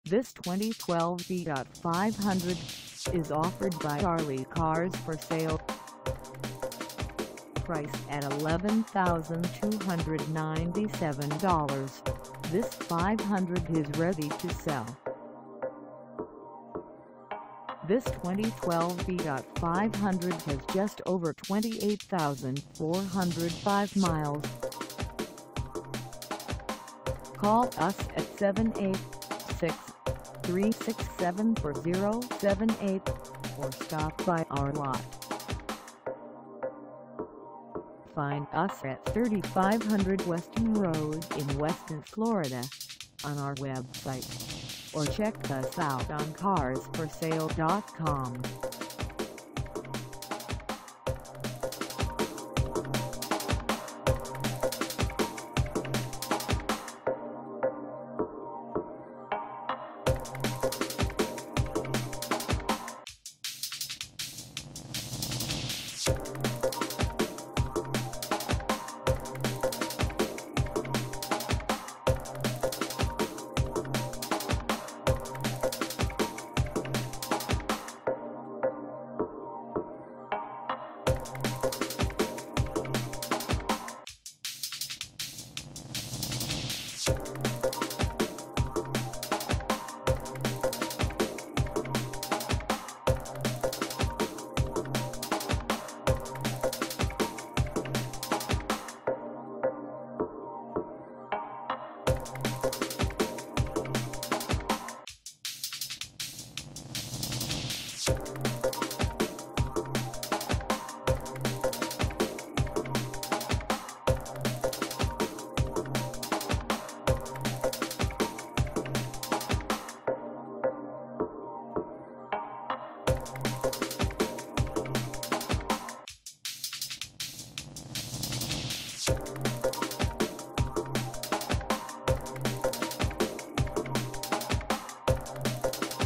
2012 FIAT 500 for sale in WESTON, FL 33331 at the Ali Cars F
This 2012 FIAT 500 is for sale in WESTON, FL 33331 at Ali Cars For Sale.
Engine: I4 1.4L I4
Transmission: Automatic 6-Speed
Color: White
Mileage: 28405
Contact Ali Cars For Sale 3500 Weston Rd WESTON, FL 33331 to test drive this 2012 FIAT 500  today.